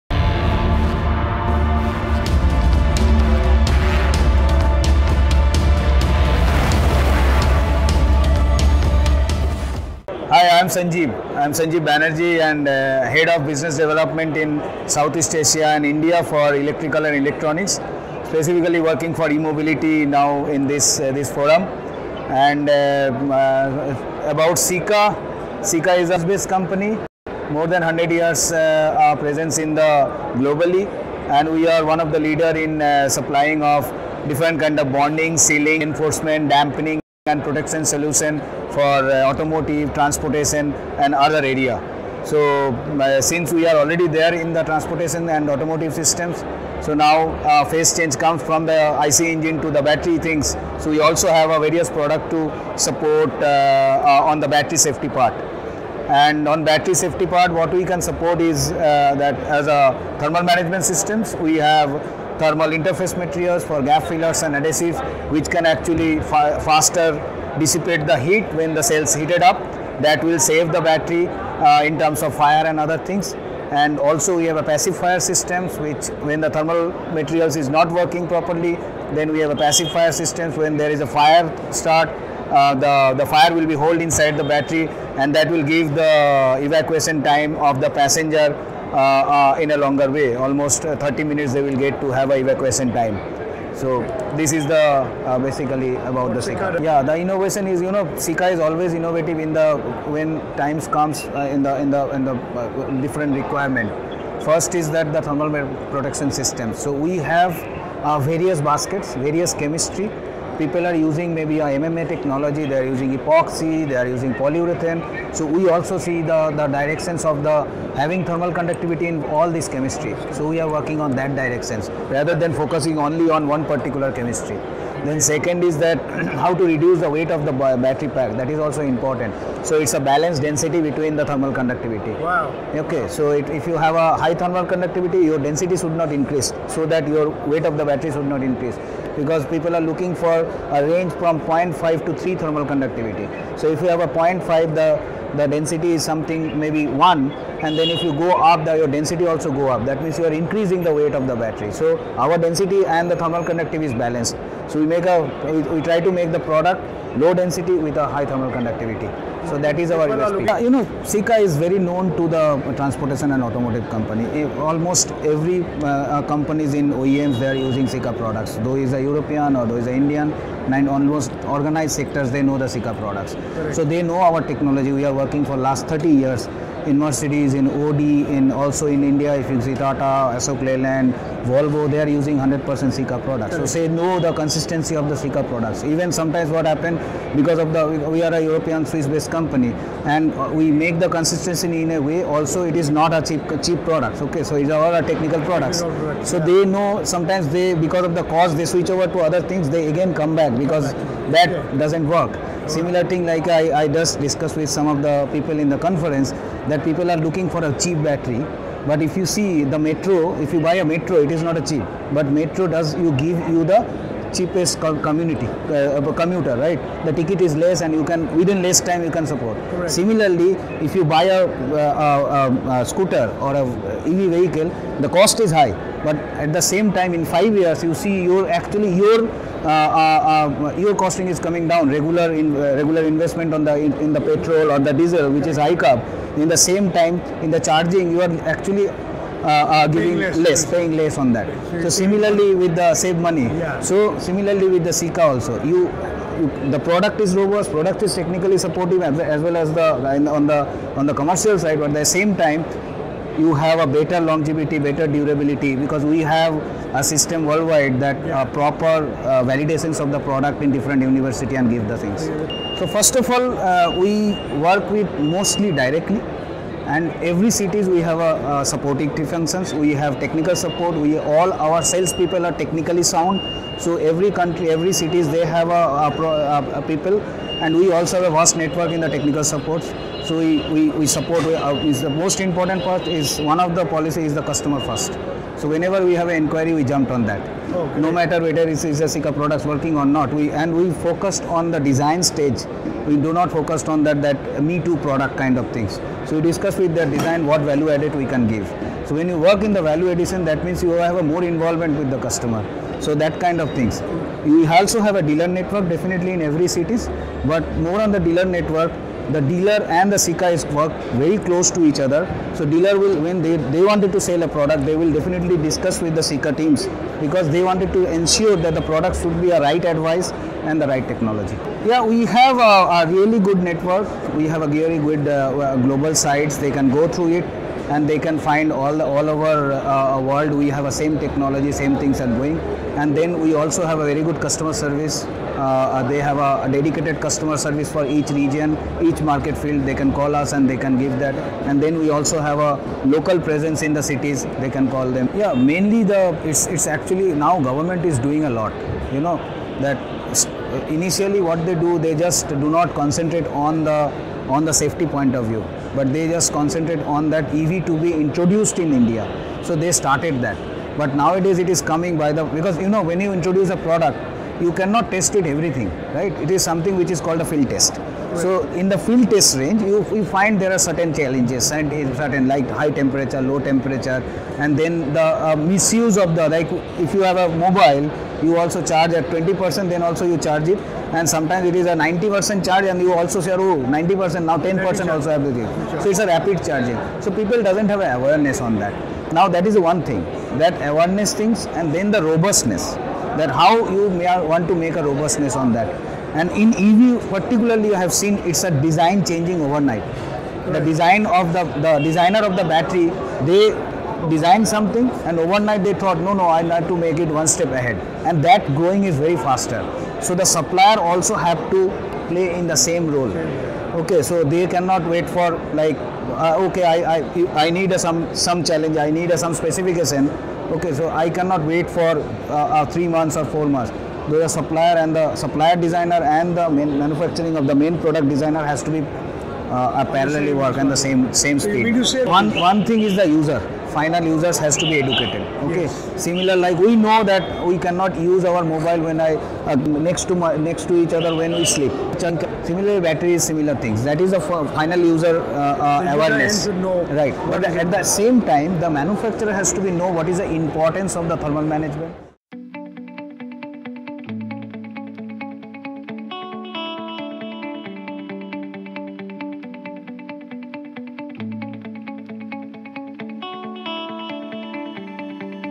EV Showcase of Sika AG with Sanjib Banerjee, Head BD India and SE Asia, Sika India
EV Showcase of Sika with Sanjib Banerjee, Head BD India and SE Asia, SIKA INDIA

Mr Banerjee shares .

'Sika has been relentlessly working for the last 30+ years with global auto makers for sealing, bonding and dampening solutions in transport and automotive systems.

Sika has recently launched battery safety and thermal management systems to save batteries from overheating and fire. Sika's Pacifier systems can help save lives incase of an unfortunate EV Battery Fire

The innovation lies in lightweighting of the battery without losing on quality and safety.

Low density with higher conductivity - An Innovation by Sika